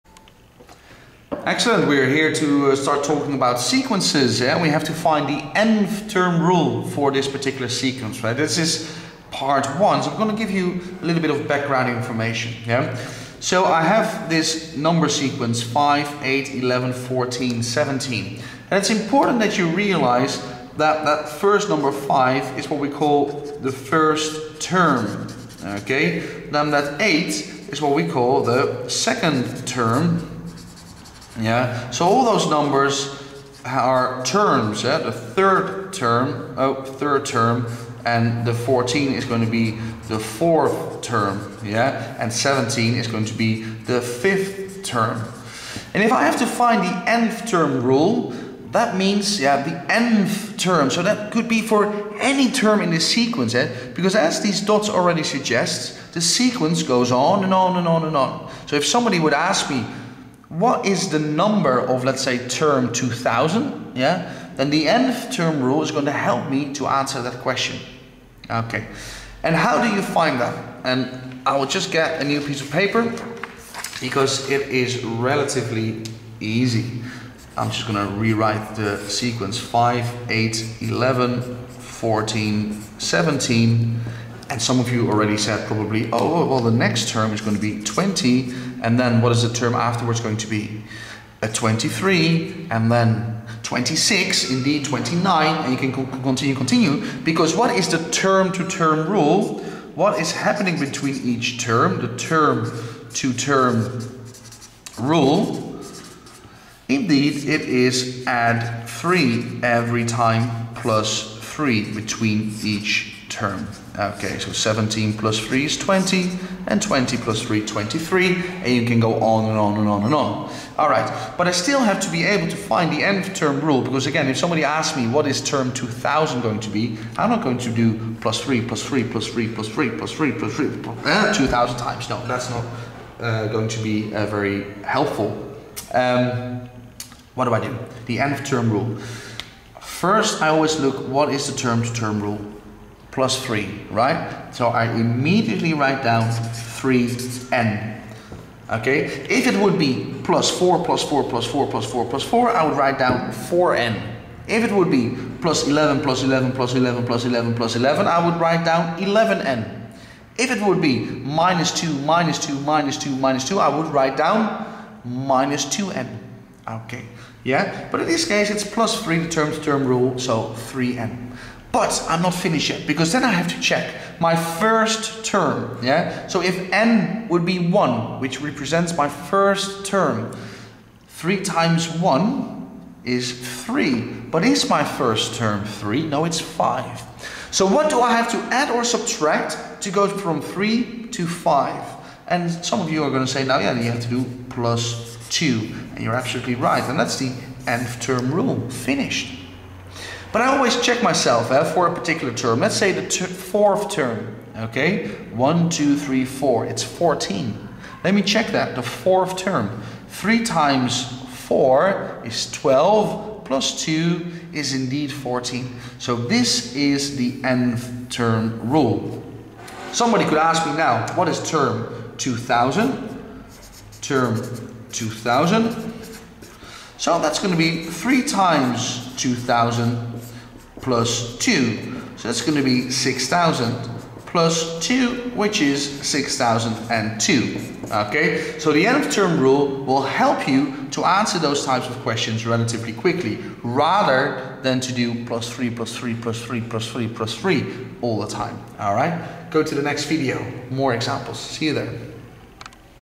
What are sequences 1 - Maths Help - ExplainingMaths.com GCSE IGCSE Maths
Learn step by step how to find the nth term rule of a sequence by looking at example questions.  After studying this video sequences will never trouble you again!  Check for more maths help and FREE resource, activities and worksheets. This maths video will explain this IGCSE and GCSE mathematical topic to you by providing example questions.  You can use these maths activities for your math revision to do better on your IGCSE GCSE exam paper.